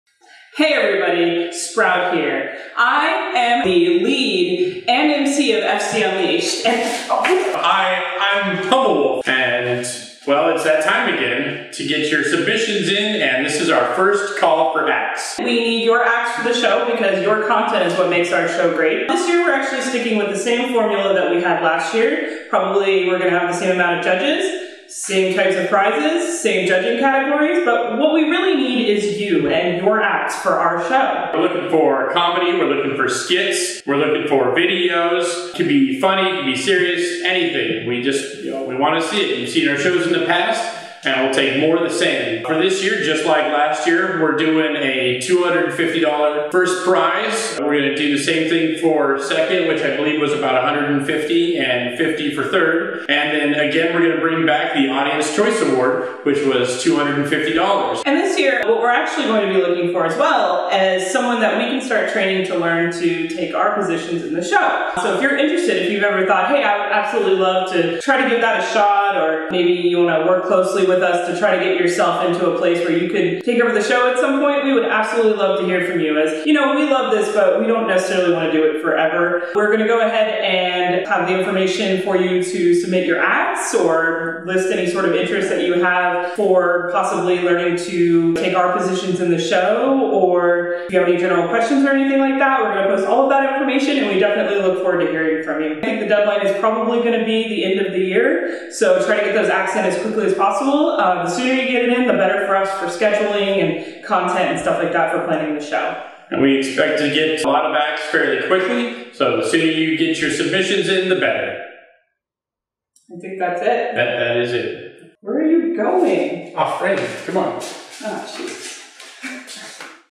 FC 2013 FC:Unleashed Call for Acts!
Hey Folks,

It's that time again! We need your acts for our show!

This is for the FC:Unleashed variety show that happens at Further Confusion every January in San Jose, CA.

We look forward to another great year!

Also, be sure to check out furcon.livejournal.com for more information on submitting an act or check out our twitter

Deadline for act submissions is 12/31/12 at midnight PST! :)

Thanks!